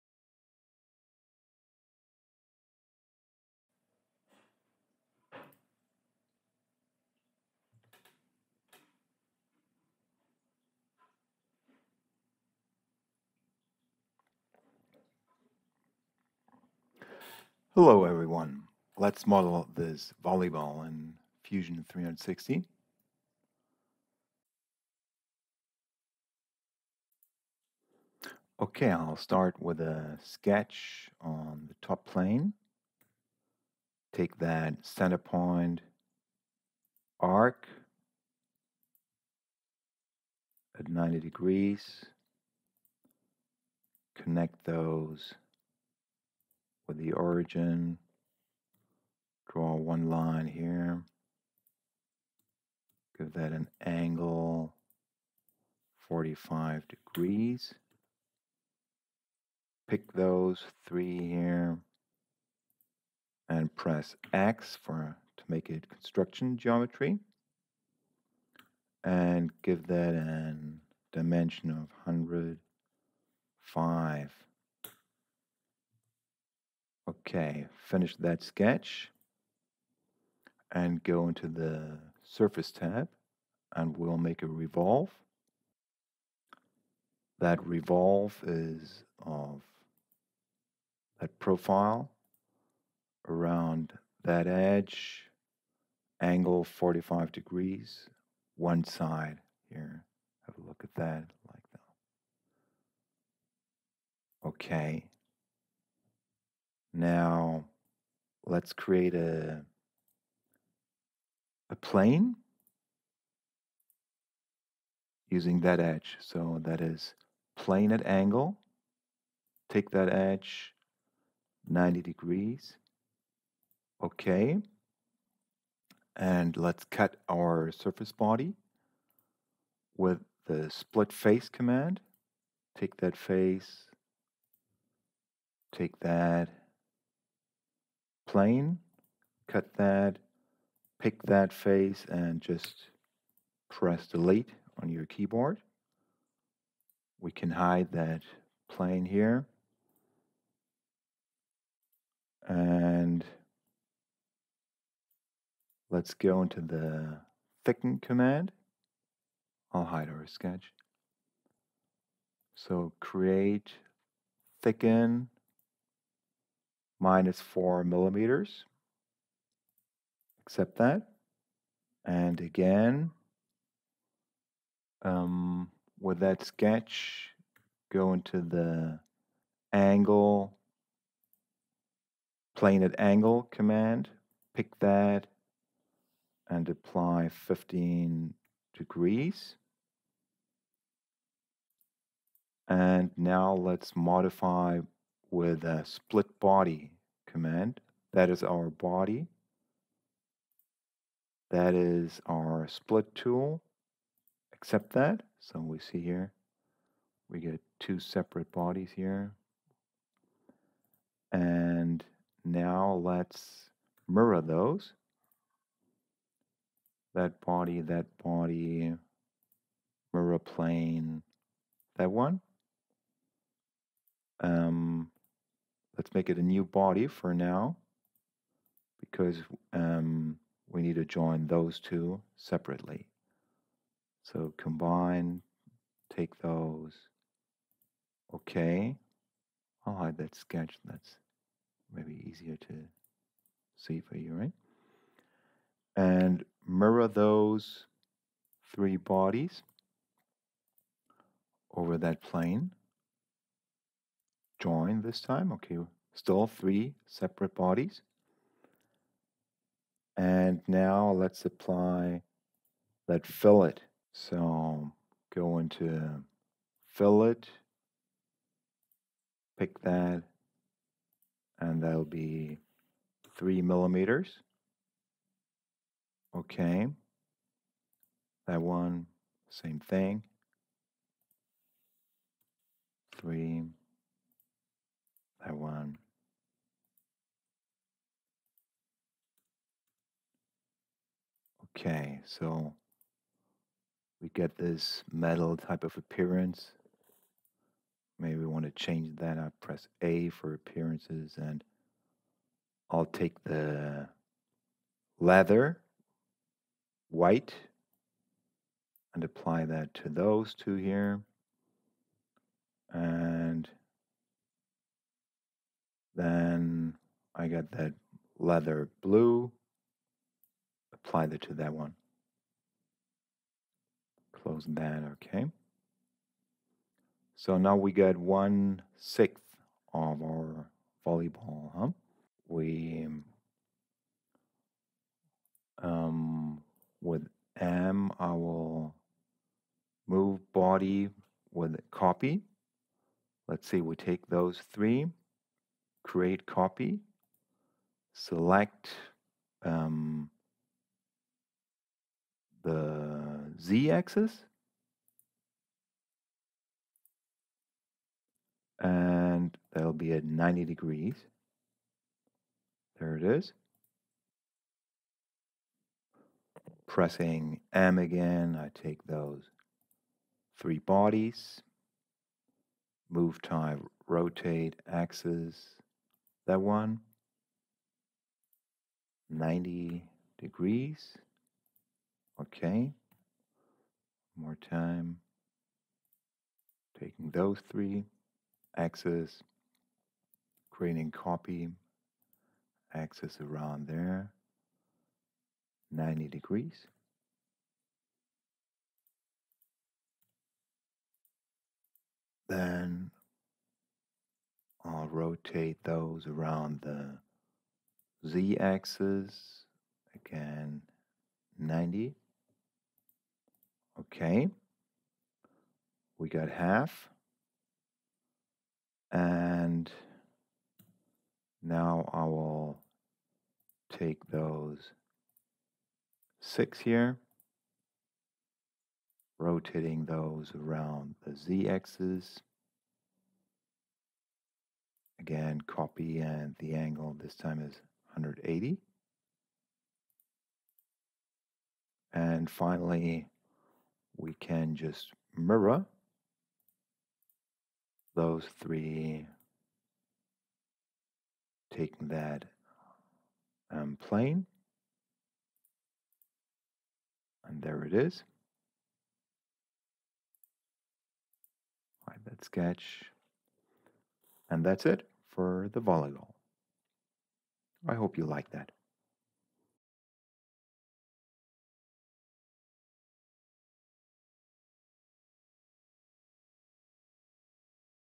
Master Volleyball design in Fusion 360 - Here's How!
In this fusion 360 tutorial, I'll show you how to model a volleyball using  basic surface commands like revolve and split face. I will then switch to solid commands, apply appearances and rotate and mirror to finish the design.

If you're interested in Fusion 360 for creating 3D objects, then this tutorial is for you! By the end of this tutorial, you'll be able to create a split face volleyball using Fusion 360. So mark the date in your calendar and join us for this fun fusion 360 tutorial!